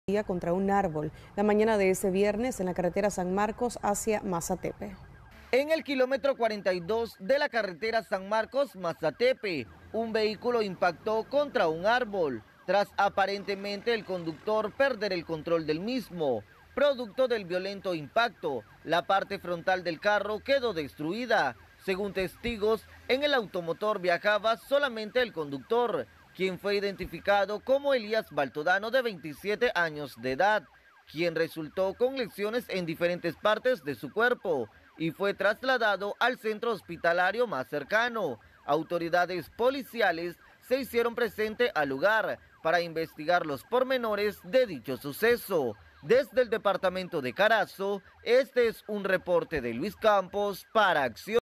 Hombre impacta con su camioneta hacia un árbol en la carretera San Marcos
El joven de 27 años quedó con varias lesiones y fue trasladado al centro de salud más cercano de la zona.